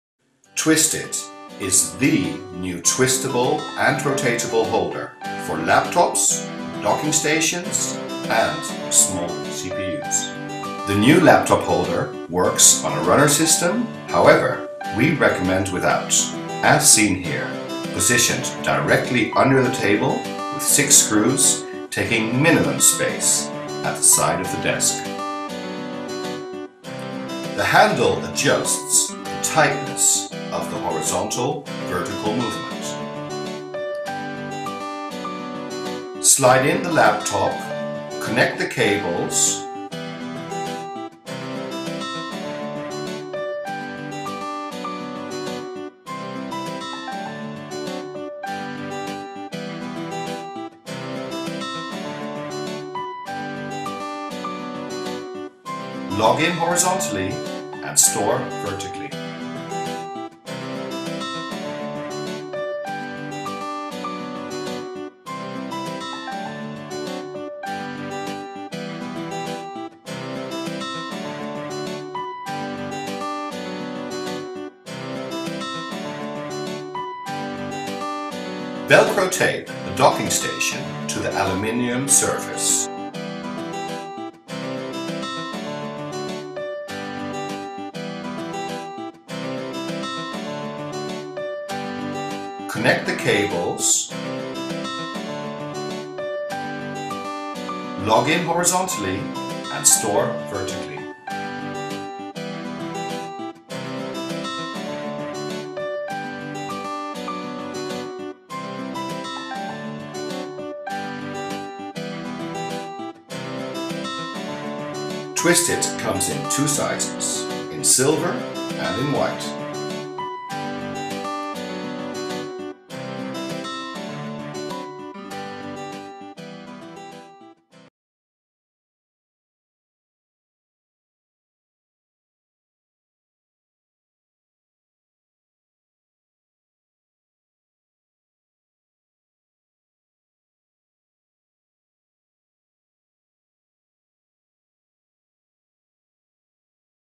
Gotessons Laptophouder TwistIT Witteveen
Gotessons Laptophouder TwistIT voor onder het bureaublad van Witteveen Projectinrichting, Ouderkerk a/d Amstel (regio Amsterdam). Zie ook ...